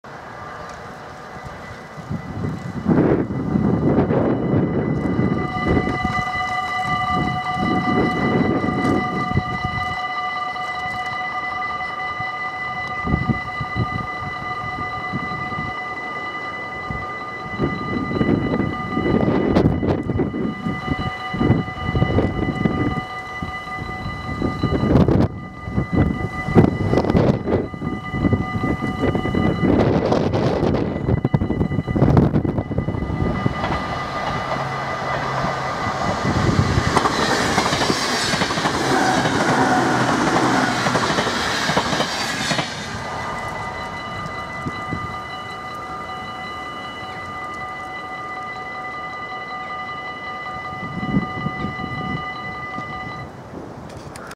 Spoorwegovergang Deventer Colmschate / Level Crossing/ Passage a Niveau/ Bahnübergang
De Intercity naar Enschede passeert de Stationsweg

Le Intercity a destination Enschede passe un passage a niveau de Stationsweg

The Intercity  to Enschede passes the Stationsweg

Der Intercity nach Enschede fahrt durch die Stationsweg

d.d. 23 december 2014